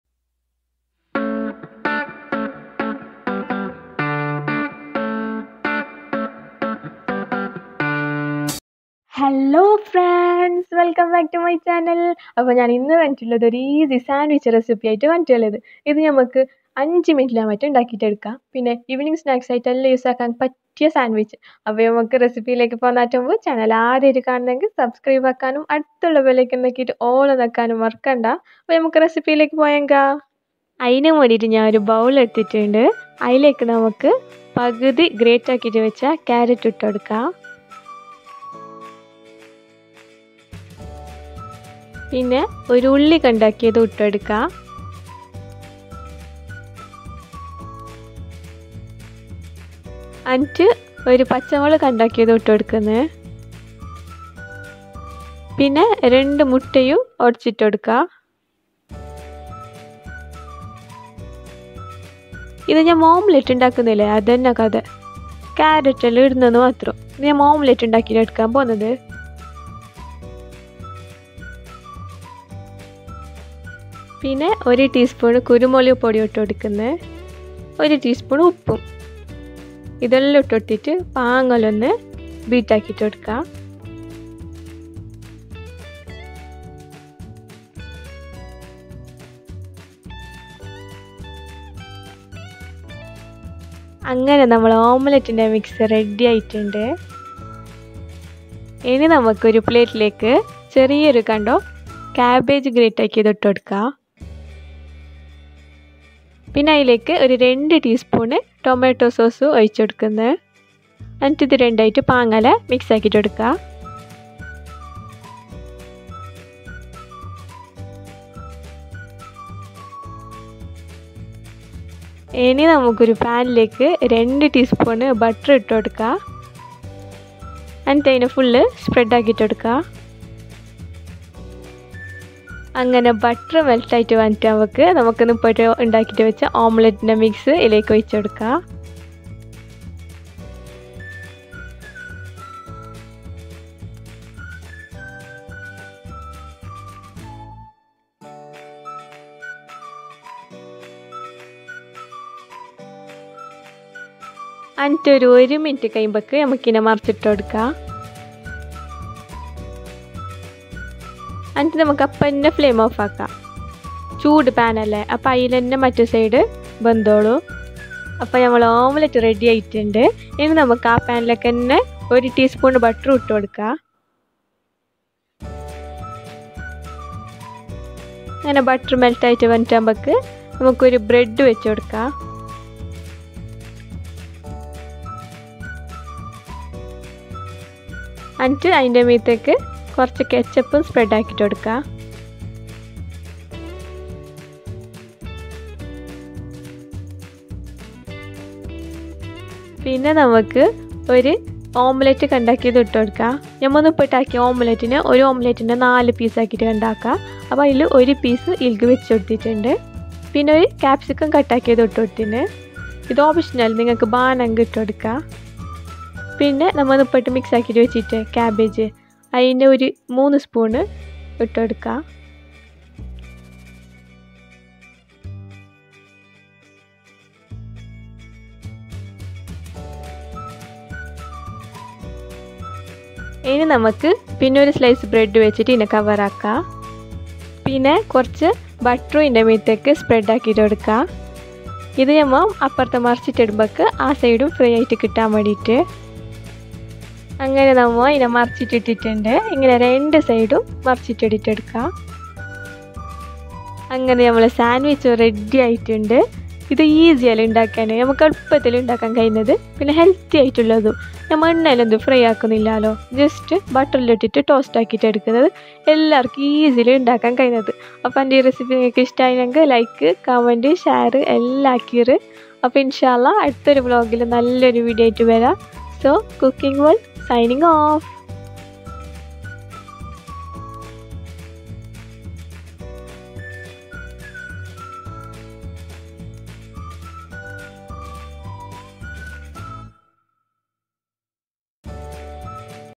Sandwich Recipie/Easy 5 minute sandwhich recipie
Ingredients
Carrot
Onion
Green chilly
Eggs
Pepper pwdr
Salt
Butter
Bread
Cabbage
Tomato ketchup
Capsicum